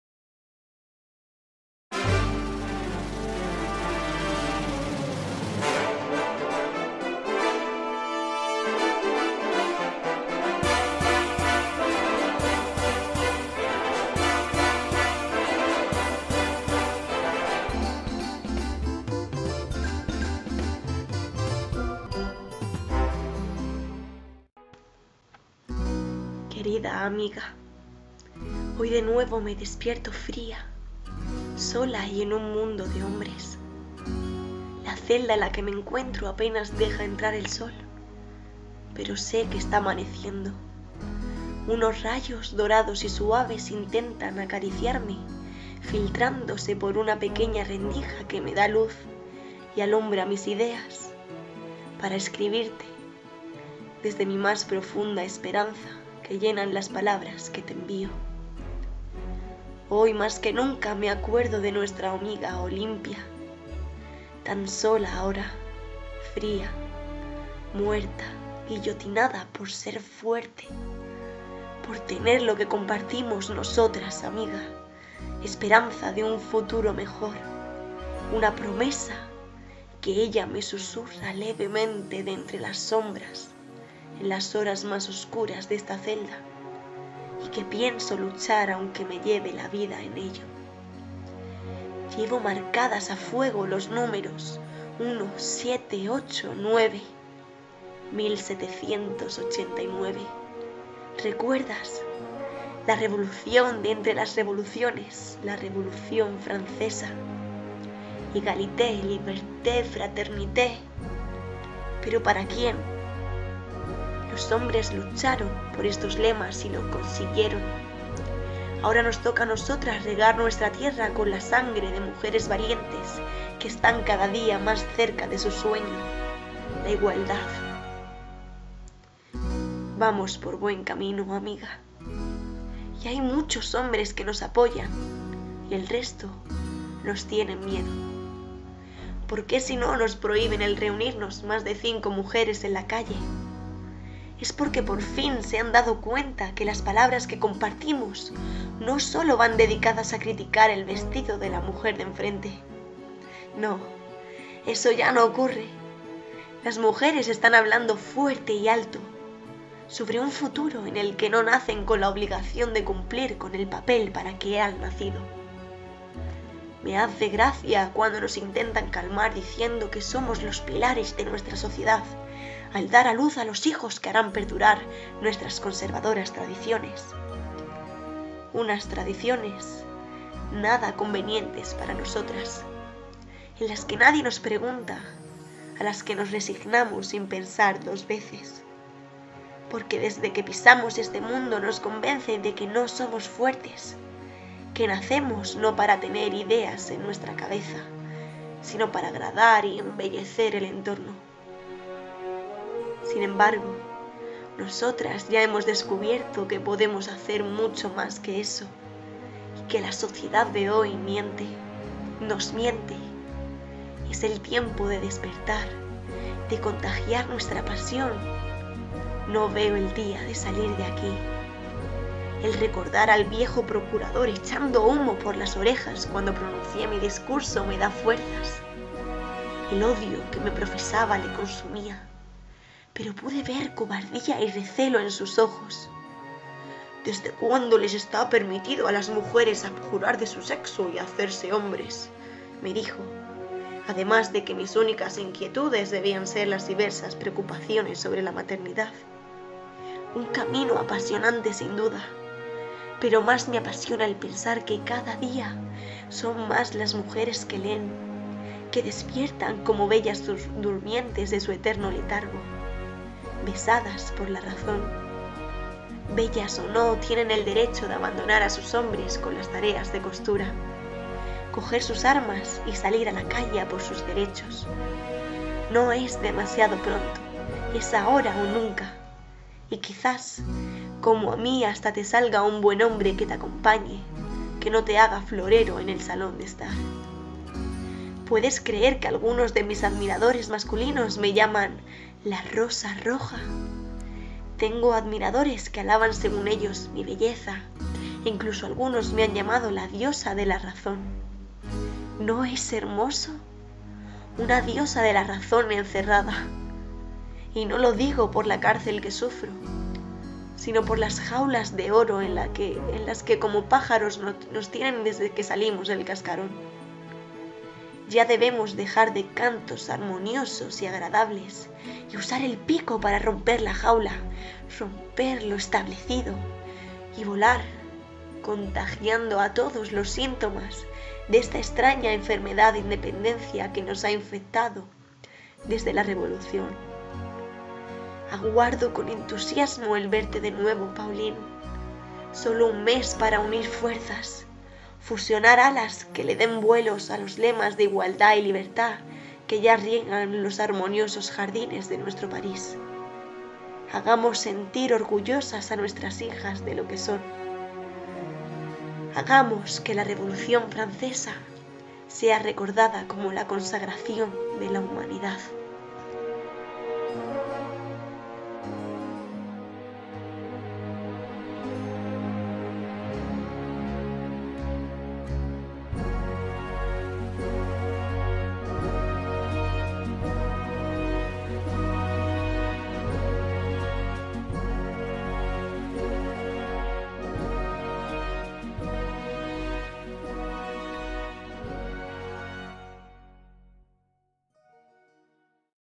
Carta: Claire Lacombe, la rosa roja de la revolución francesa
Bueno chicos, tenía ganas de hacer algo diferente y como es una mujer a la que he estudiado en mi carrera y que realmente me gustó, he decidido dedicarle este vídeo resumiendo en una carta ficticia sus experiencias, sus miedos, su fuerza y sobre todo su esperanza. Una lucha sin igual que dio pie a los primeros comienzos de la libertad de las mujeres.
P.D: Por si no se ha notado en el video, la revolución francesa es mi momento histórico favorito y realmente me apasiona.
Un beso muy grande y gracias a todos.